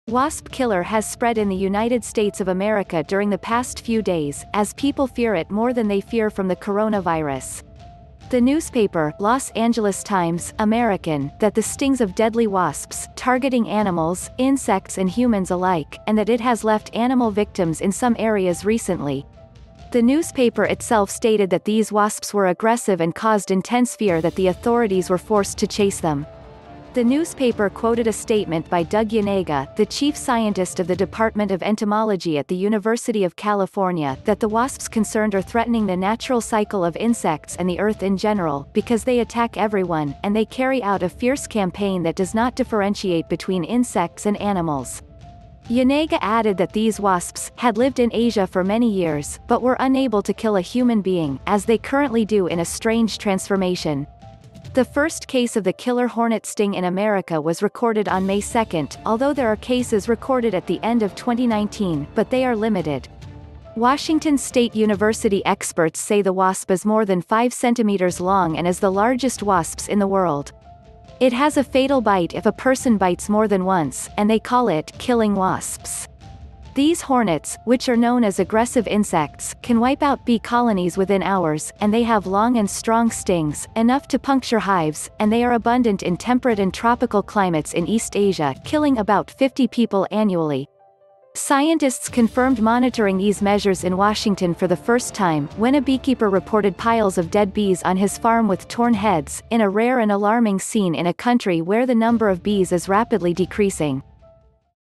Killer Hornet ! Asian giant hornet.
The Asian giant hornet , including the color form referred to as the "Japanese giant hornet"2.3, is the world's largest hornet.